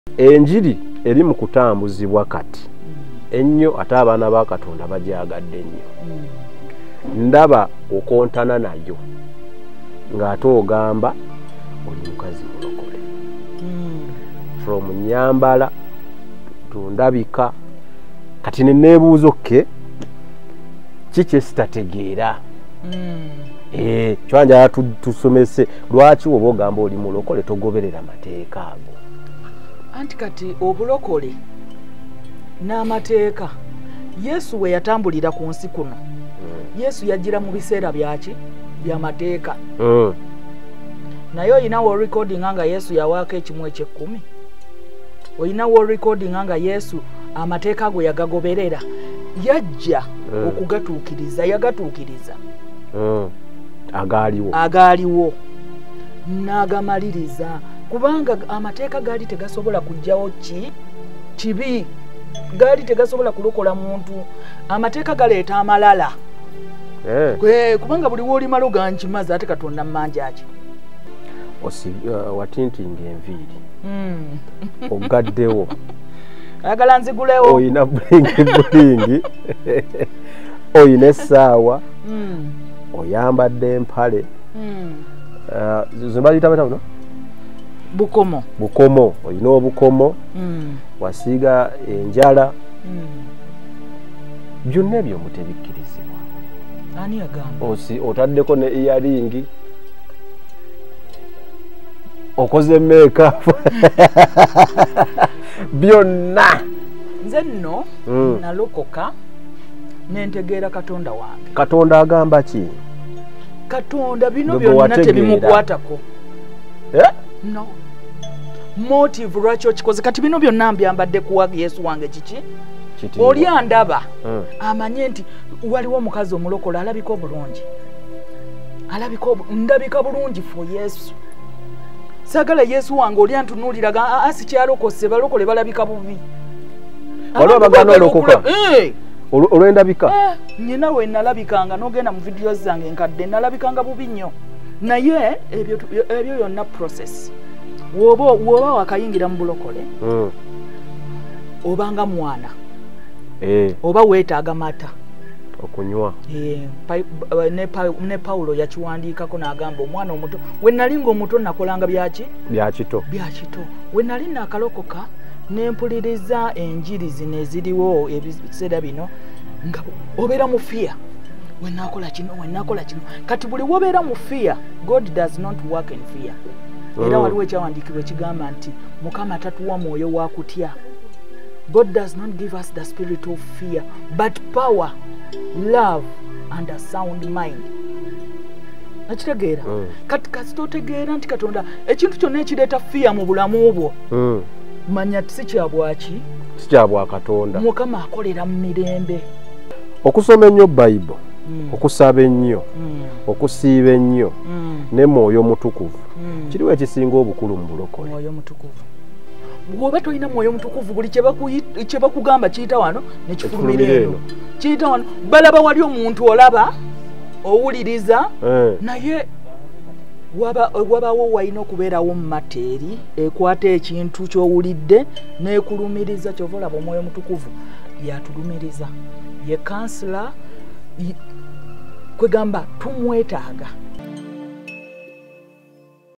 Coming soon....
Mukyala Faridah eyali omusiramu NALOKOKA Anyonyola olugendo lwe nabiki byeyayitamu munsi yomwoyo.